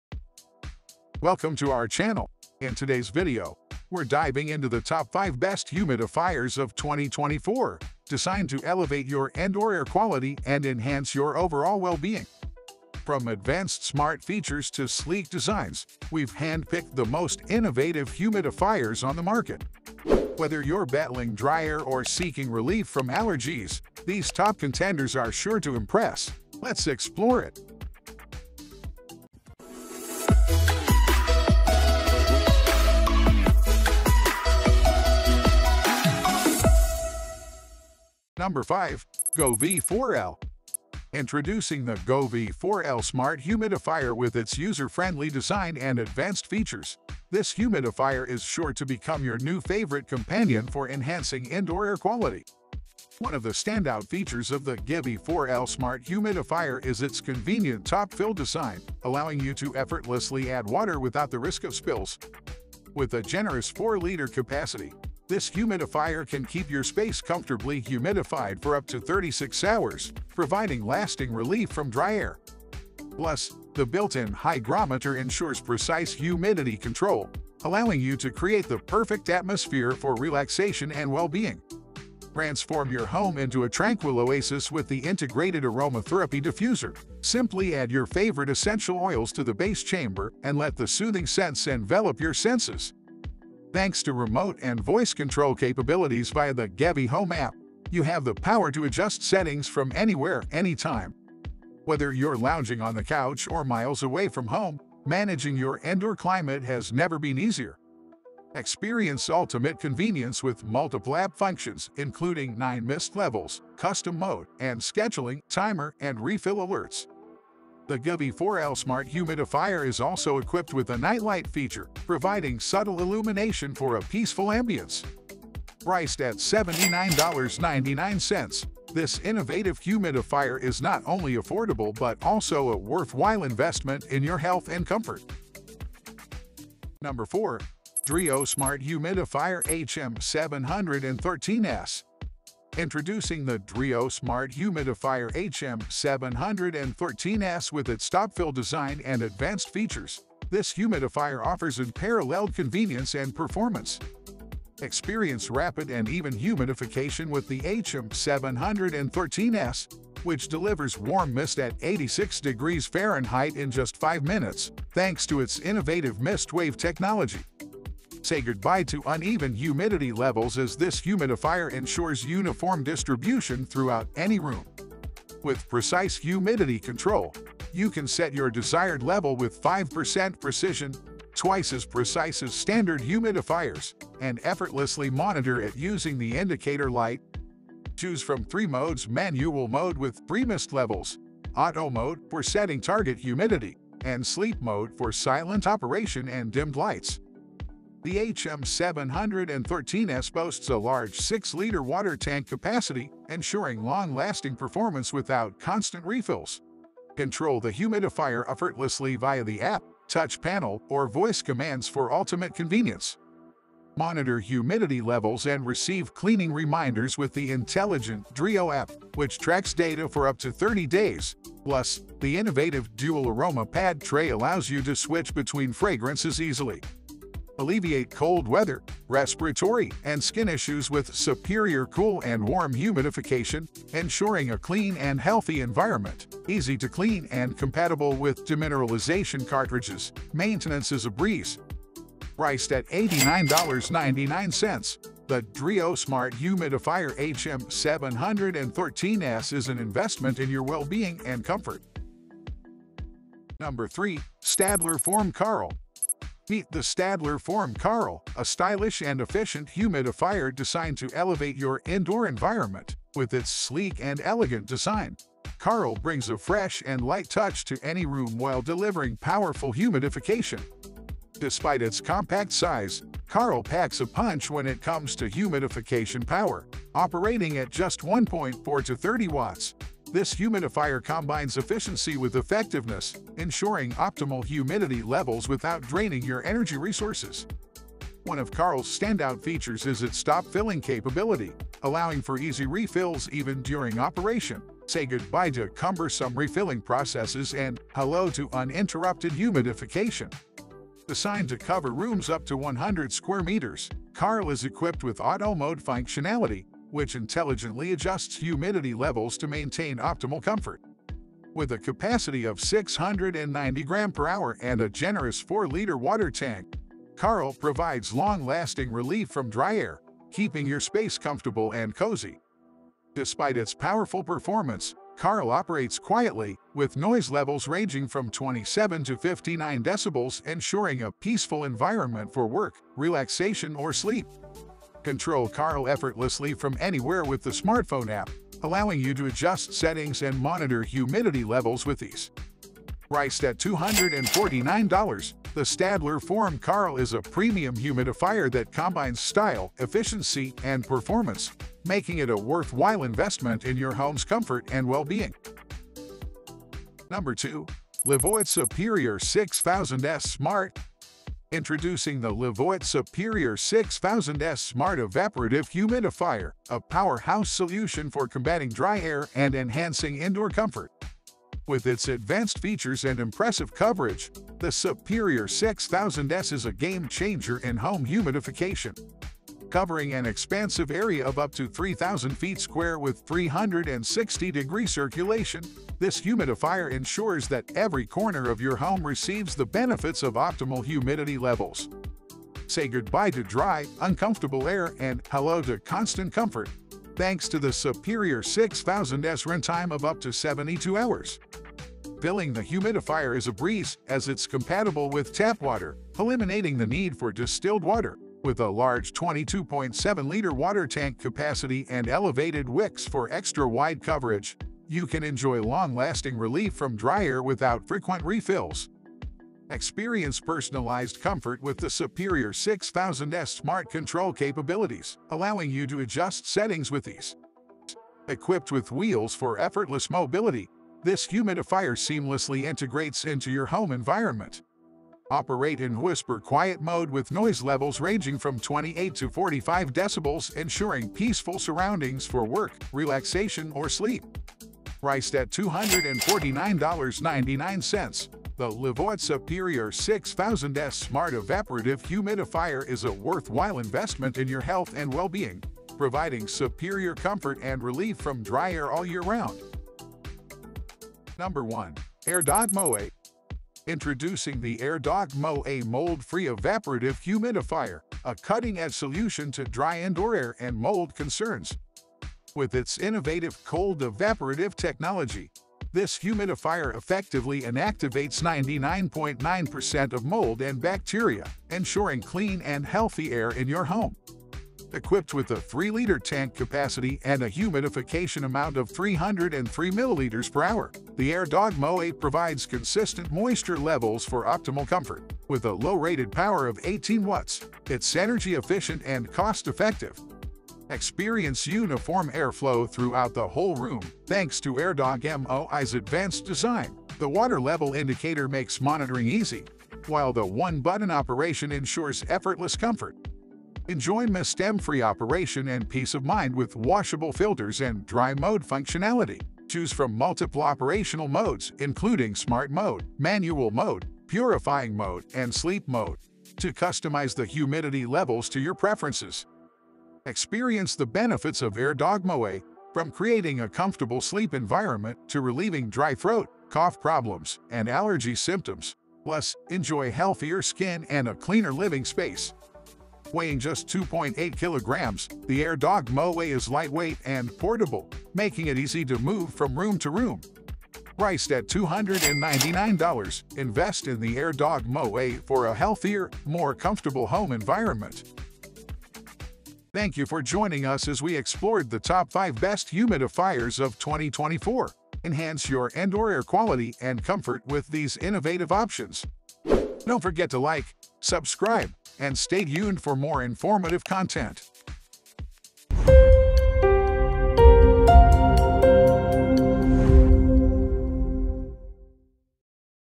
Best Humidifiers 2024 - Trendy Humidifiers for a Healthier Home
00:00 - Introduction

►00:38 - 5: Govee 4L
►02:17 - 4: Dreo Smart Humidifier HM713S
►04:13 - 3: Stadler Form Karl
►05:57 - 2: Levoit  Superior 6000S Smart
►07:41 - 1: Airdog MOI

07:20 - Conclusion
Discover the top 5 best humidifiers of 2024 in our latest YouTube video! From sleek designs to advanced features, we've curated a list that promises to enhance your indoor air quality and promote better health. Don't miss out on finding the perfect humidifier for your home or office space!